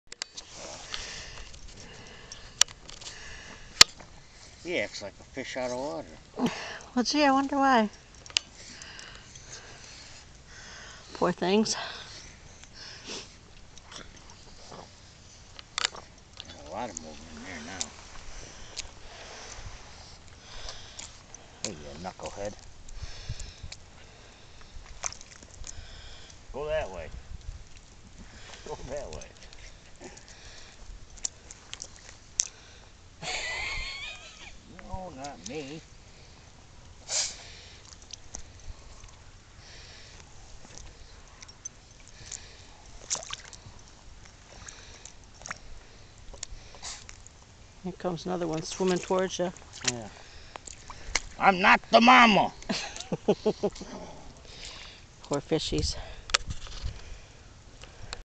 Big Minnow.AVI
Minnows were popping up out of small fissures in the ice. This was the biggest little guy we had seen. We tried scooching them back to a hole so they could go back in, but every time we got near the holes, more water poured out & it just made matters worse. Eventually just had to give up trying to help.